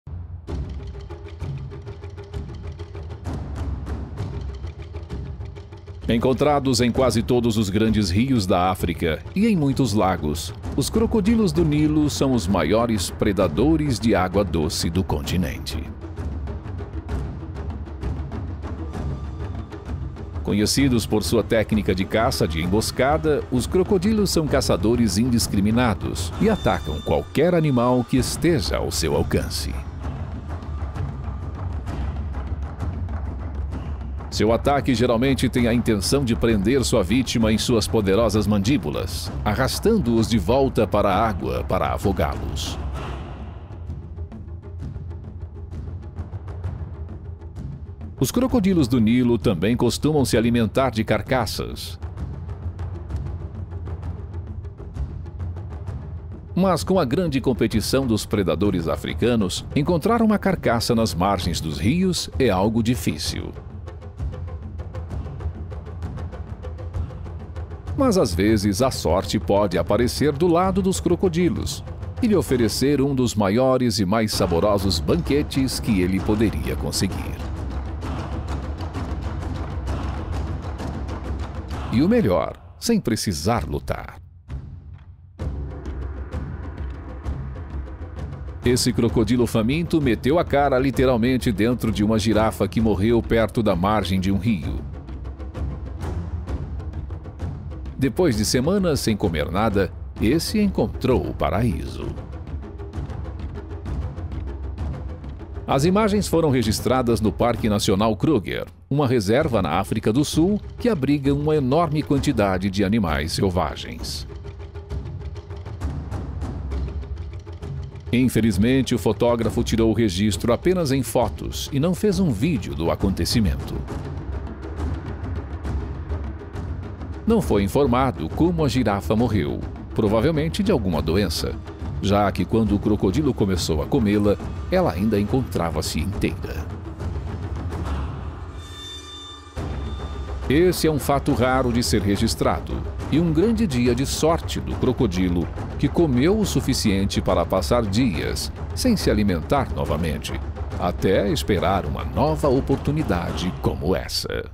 FOI POR ISSO QUE ESSE CROCODILO SE DEU BEM
O crocodilo faminto meteu a cara literalmente dentro de uma girafa que morreu perto da margem de um rio veja nesse video.